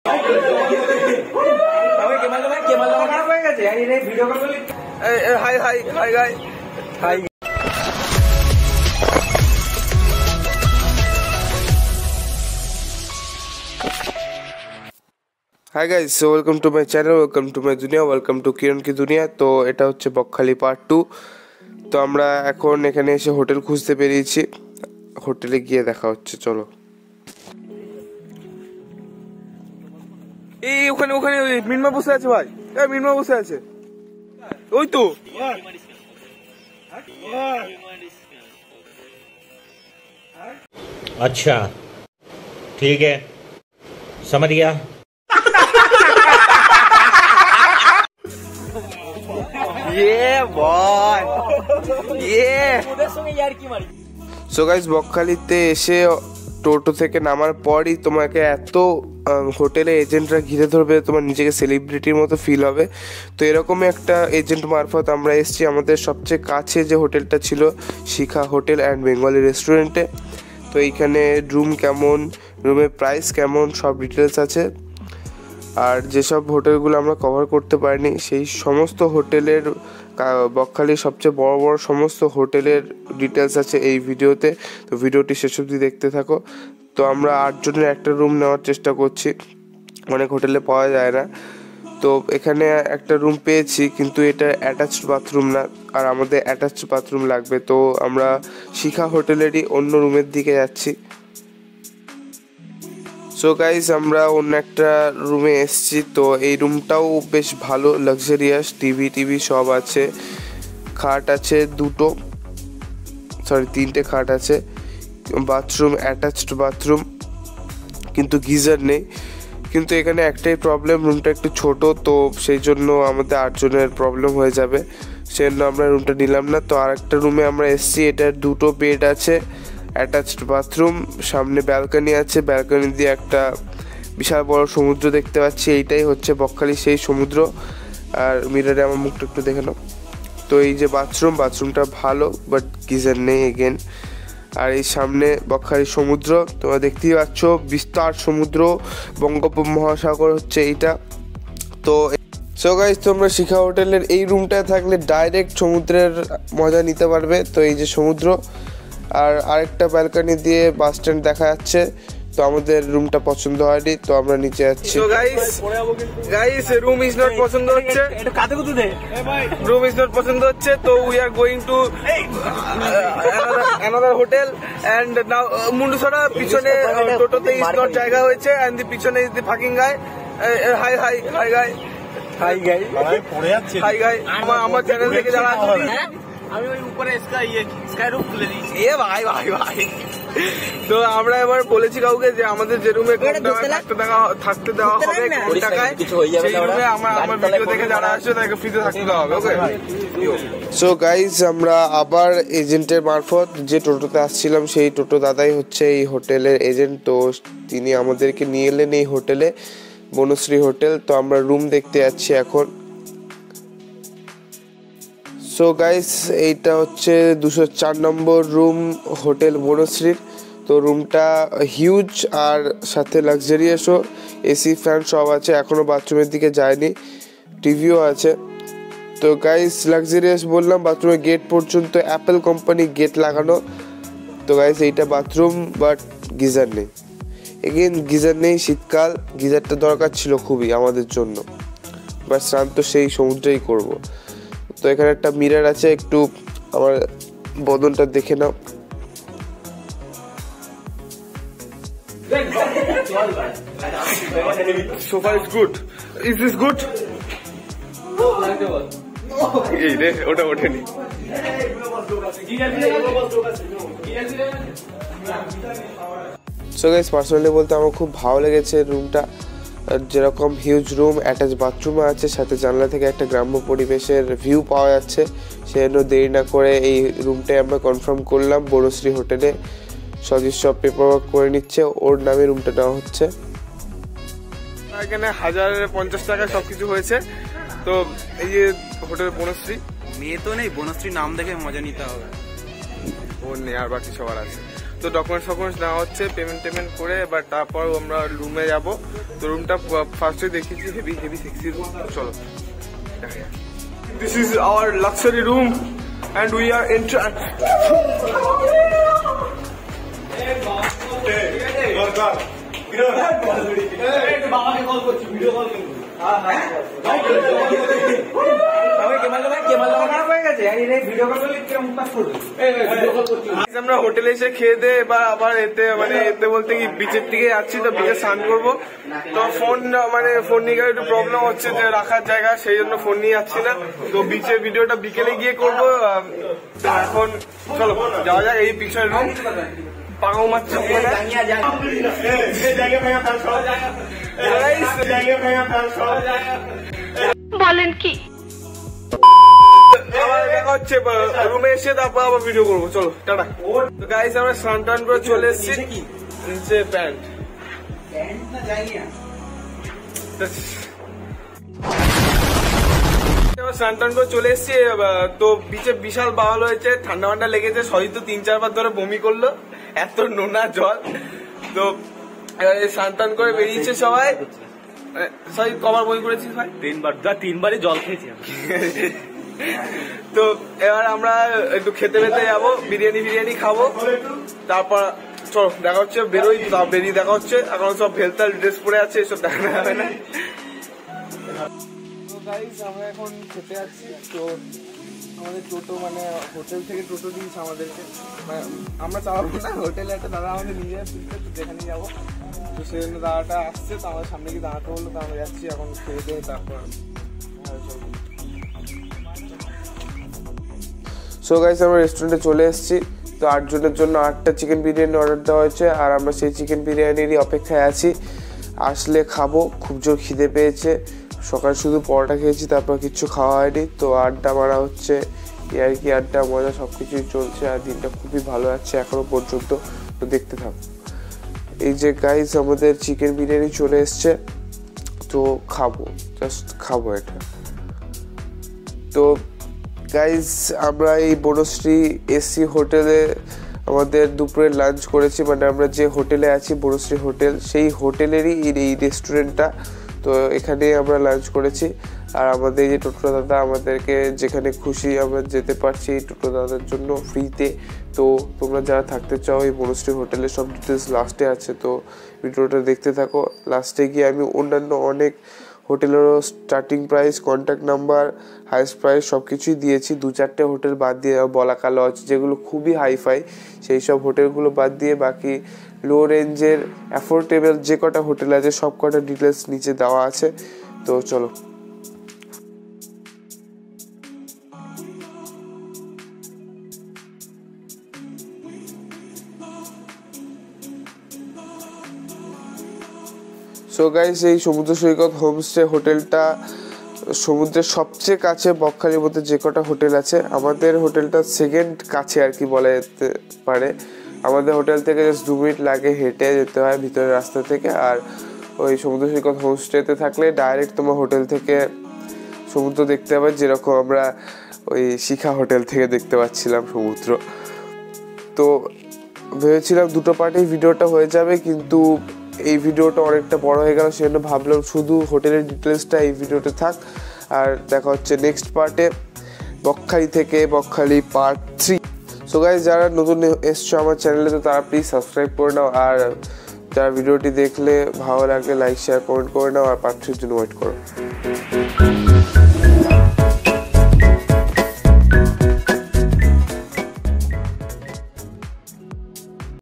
Bakkhali Hotel : Lowest Hotel price in Bakkhali : Details about all hotels in Bakkhali : Part-2
Hiiii guys
welcome to my duniya
welcome to Kiran ki duniya

I'm Kiran 😀

My first travelling vlog
How to go from Kolkata to Bakkhali at low cost has a complete description in this video